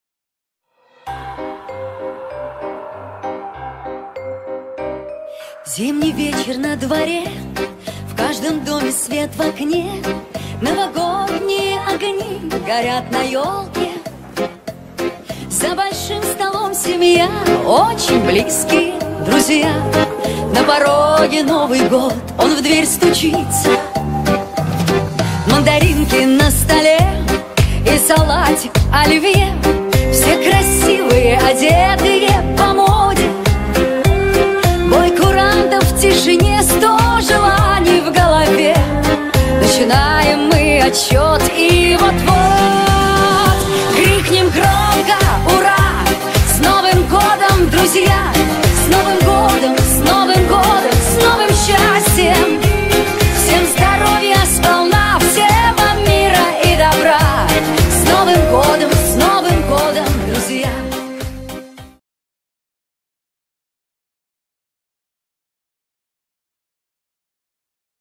Музыкальное Поздравление с Наступающим Новым 2022 Годом!!
С наступающим Новым Годом,мои дорогие!!😍😍😍🙏🐯🥂🍾🌲🎉🌞🌹😘🎁🎁🎁
Будьте Здоровы,Радостны и Благополучны!
Моя огрОмная благодарность за замечательные футажи​ Footage from Kati Титову.